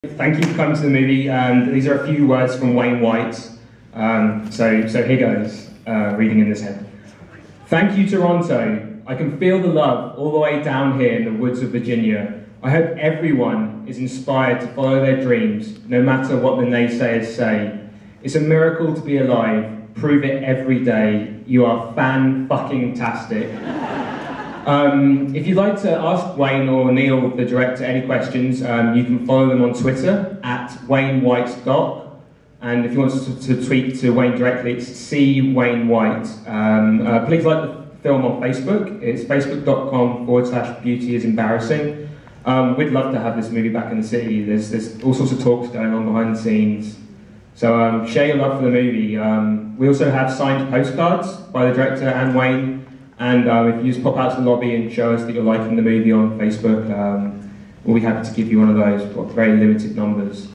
'Beauty Is Embarrassing' Hot Docs 2012 Q&A
This Q&A for the film 'Beauty is Embarrassing' took place during the Hot Docs Documentary Film Festival 2012.